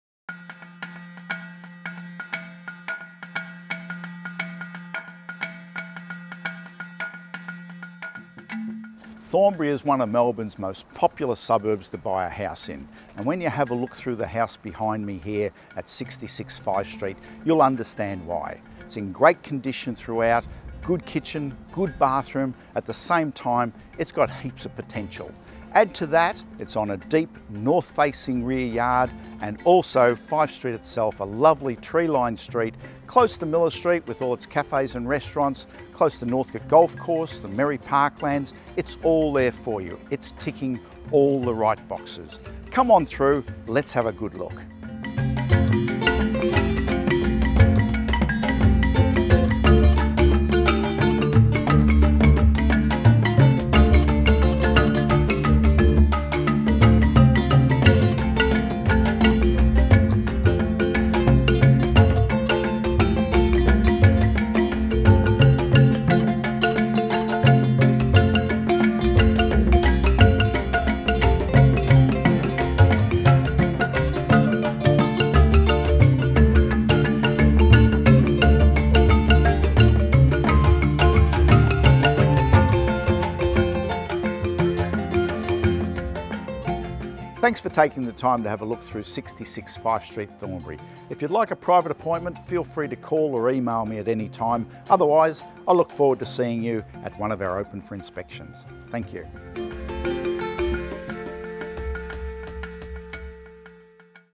66 Fyffe Street, Thornbury For Sale by Peter Egan of Nelson Alexander
or call Peter Egan on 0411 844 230

Surrounded by quality period homes, this Californian classic offers an immediately comfortable environment with value-adding opportunities to modernize, extend or possibly start anew (STCA). Current floorplan includes two bedrooms (large front master with bay window), contemporary-style bathroom and separate powder room, spacious lounge/dining area through to the updated kitchen and adjoining meals area. Add to this gas heating, polished timber floors, plus a deep north-facing backyard highlighting the property's generous dimensions and untapped potential. Complete with front off-street parking, it's a short stroll to Miller Street cafes and shops, St Georges Road trams, close to Strettle Wetland and Merri Creek trails, local parks and Bell Primary School.